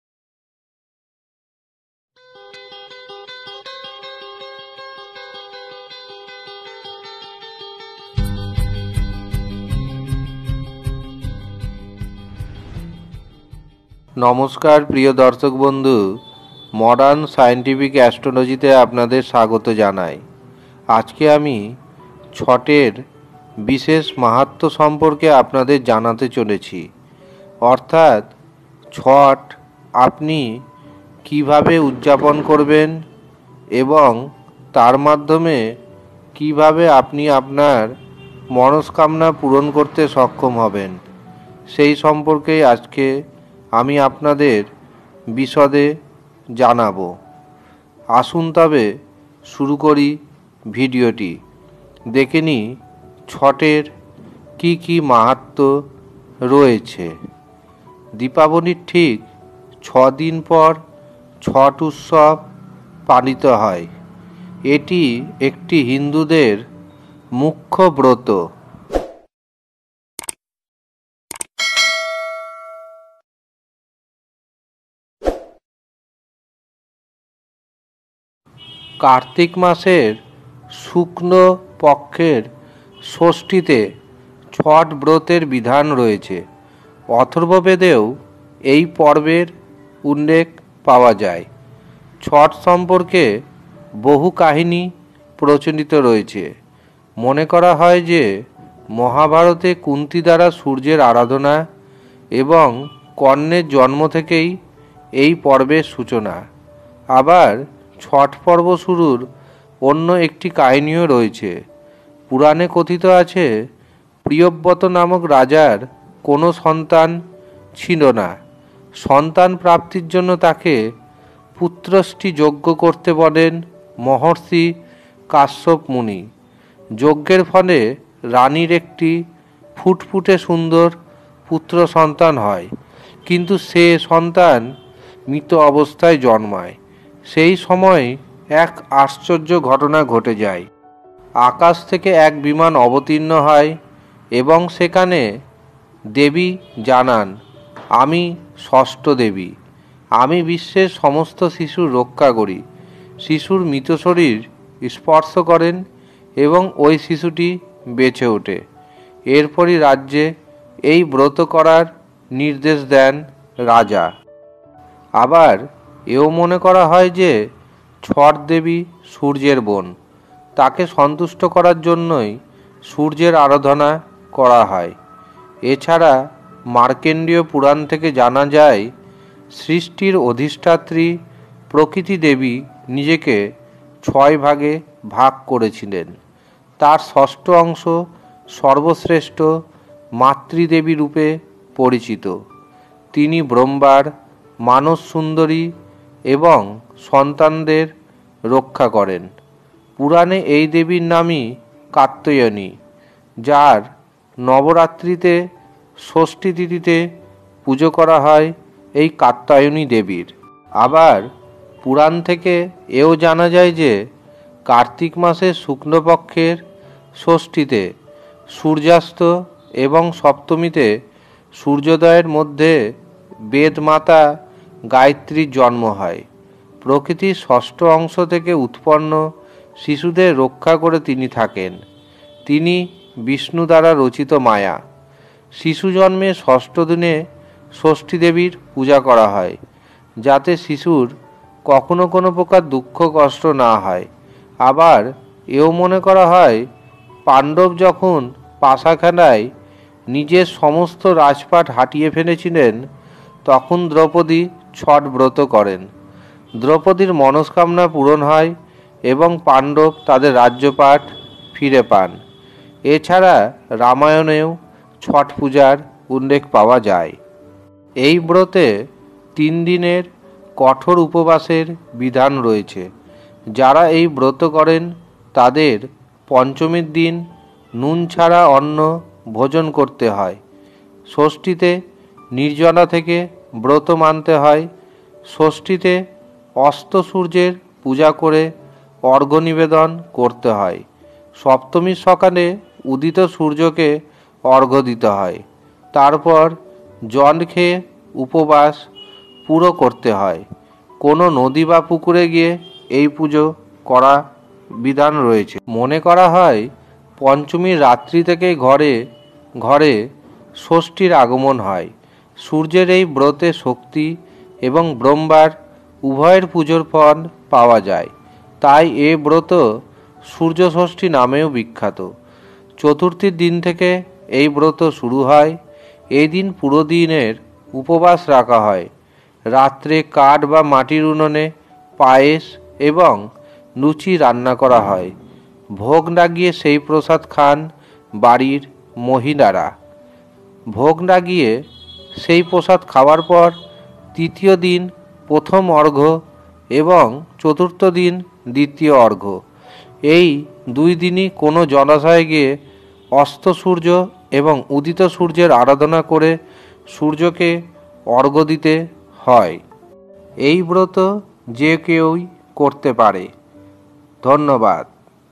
ছট পূজার মাহাত্ম্য। ছট পূজা নিয়ম জেনে নিন।ছটপূজা
ছট পূজার মাহাত্ম্য ও নানারকম ছট পূজায় যে নিয়ম রয়েছে তা সঠিকভাবে জেনে নিন

Modern Scientific Astrology সম্পূর্ণরূপে বিজ্ঞানসম্মত জ্যোতিষ ভিত্তিক You Tube চ্যানেল। এখানে জ্যোতিষ ধর্মের আলোচনা আপনারা পাবেন।।